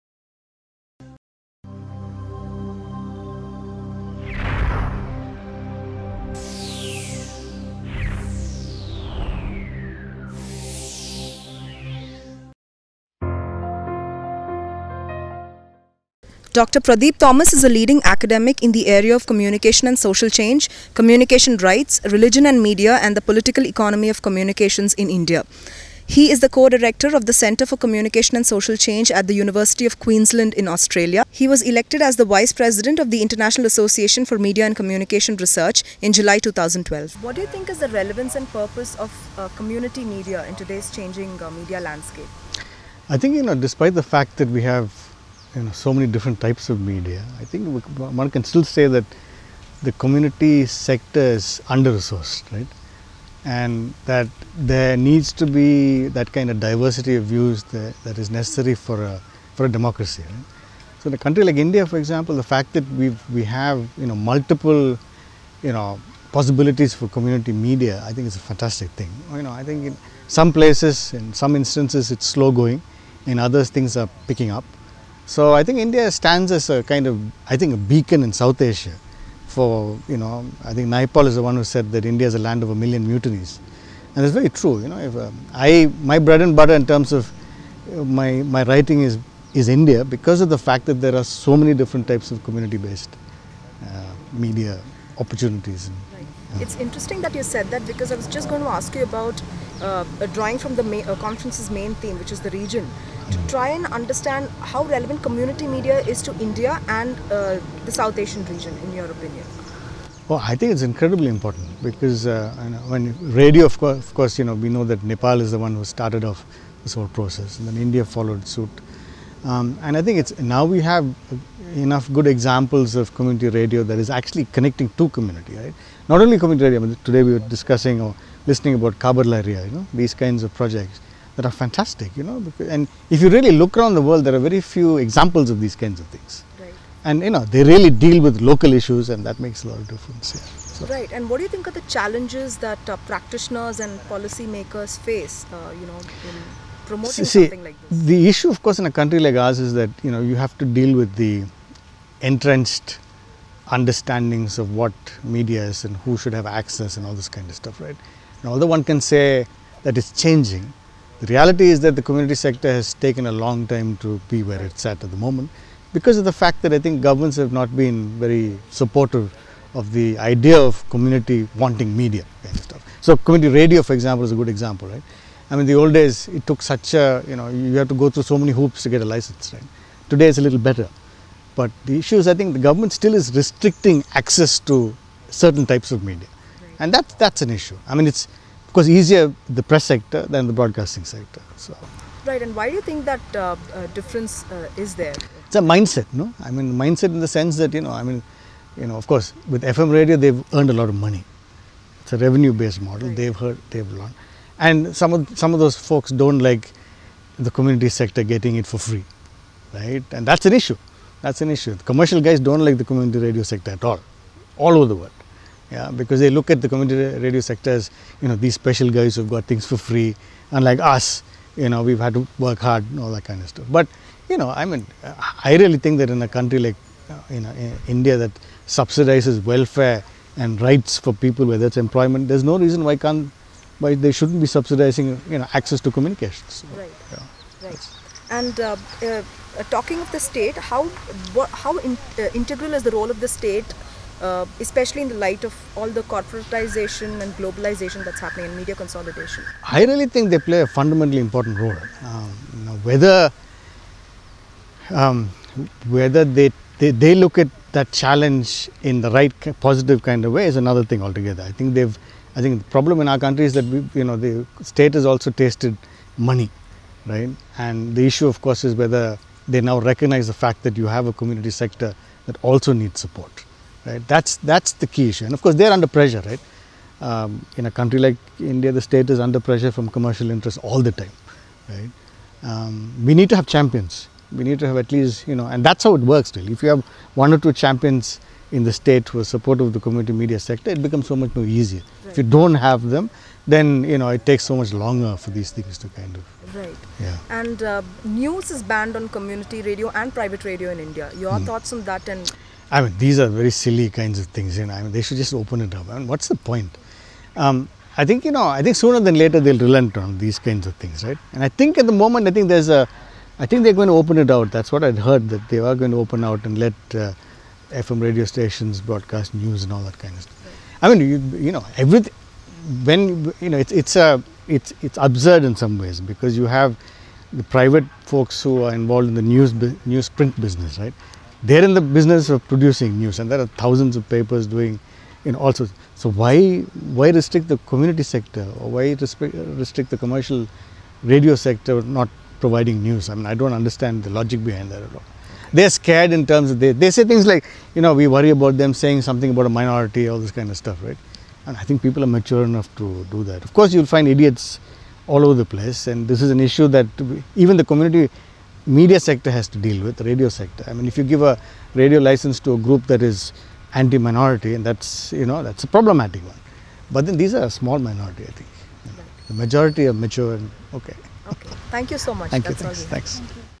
Prof. Pradeep Ninan Thomas
to know more about UNESCO Chair on Community Media.

Associate Professor Pradip Ninan Thomas is a leading academic in the area of communication and social change, communication rights, religion and media and the political economy of communications in India. He is the co-director of the Centre for Communication and Social Change at UQ. He was elected as the Vice President of the International Association for Media & Communication Research ( IAMCR) in July 2012. He has written extensively on the media and communication including four authored volumes between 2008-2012. He completed a trilogy on the media in India with the publication of the book Digital India: Information. Communication & Social Change (2012). He is  on the editorial boards of of a number of journals including Media Development, Journal of Creative Communications, Journalism and Communication Monographs and the International Journal of Press/Politics and is also on the advisory board of a number of international institutes including the India Media Centre at the University of Westminster. He is on the Advisory Board of a book series with the University of Illinois Press ‘The Geopolitics of Information’ along with Prof. Dan Schiller and Prof. Yuezhi Zhao.